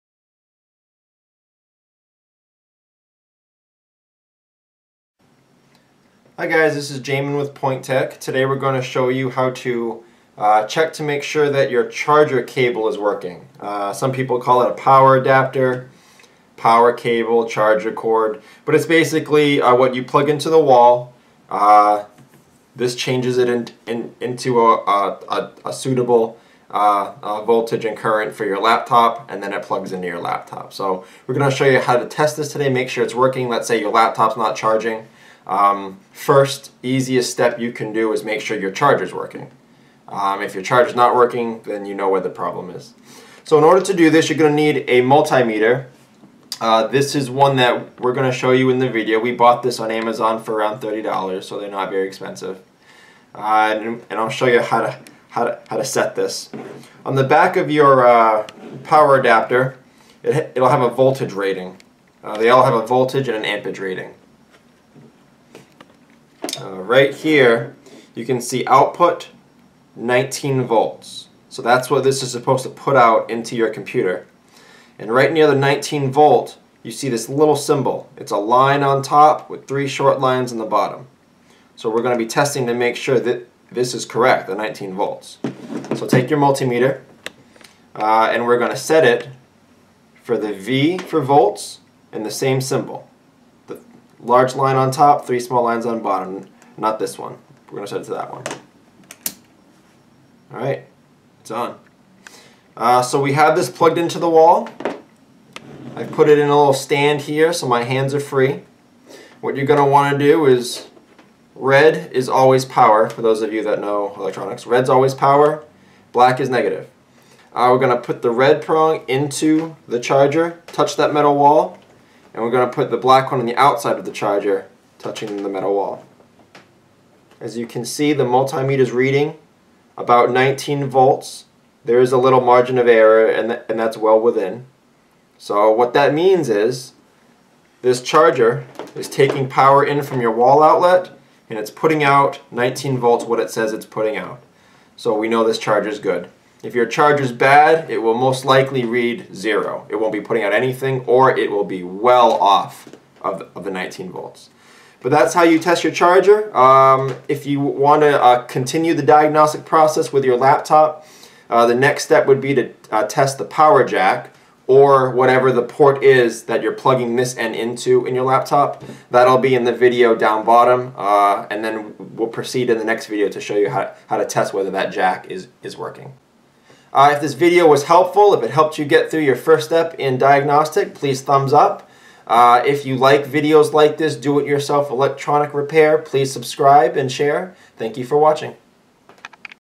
How To Test your Laptop Computer Charger Power Cord Adapter Cable
This video will show you how to test your power cord with a multimeter to make sure its functioning properly.  If your here because your computer is Dead / No Power, try the steps seen For help finding a multimeter or other computer repair tools and supplies, visit my list

FAQ's:
Q: Why didnt you test the amps?
A: Amps are the flow of power. In order to test the amps, the computer must be on and using power. That opens up risk of damage to things being tested. Therefore, I do not have videos on testing 'live power' for my viewers.

(edited 7/25)